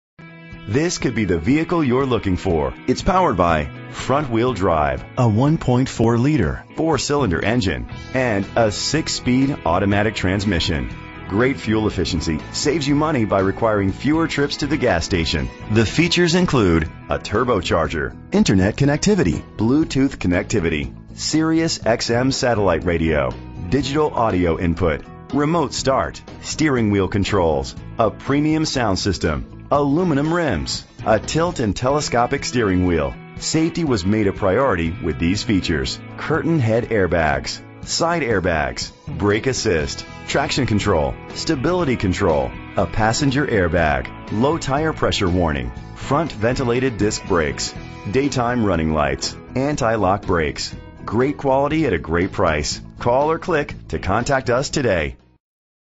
2016 Chevrolet Trax - Birmingham AL
Year: 2016
Make: Chevrolet
Model: Trax
Trim: LT
Engine: 1.4 liter inline 4 cylinder 16 valve
Transmission: 6-Speed Automatic
Color: Crimson Metallic
Mileage: 1
Address:
1400 Third Ave. North